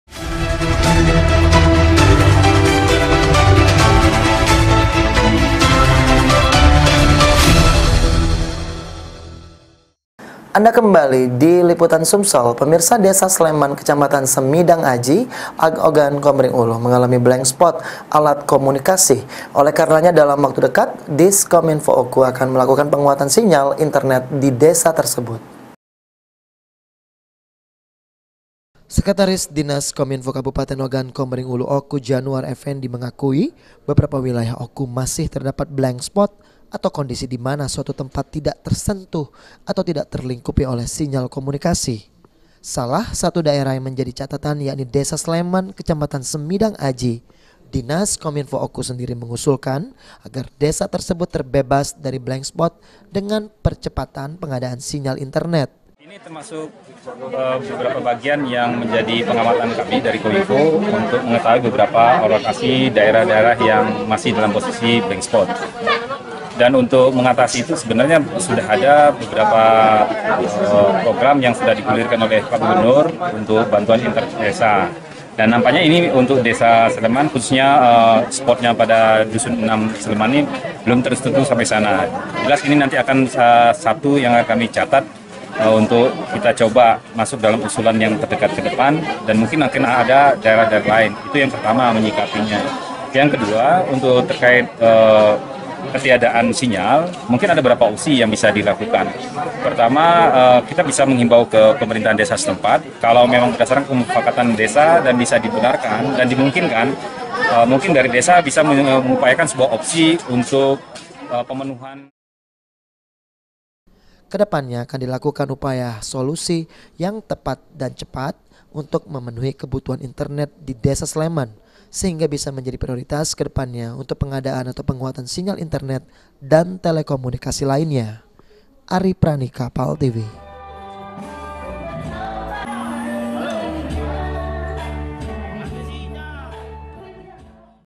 Desa Blank Spot Akan Dilakukan Penguatan Sinyal
Desa Seleman / Kecamatan Semidangaji / Ogan Komering Ulu /Mengalami Blank Spot Alat Komunikasi // Oleh Karenanya /Dalam Waktu Dekat Diskominfo Oku / Akan Melakukan Penguatan Sinyal Internet Di Des Tersebut ///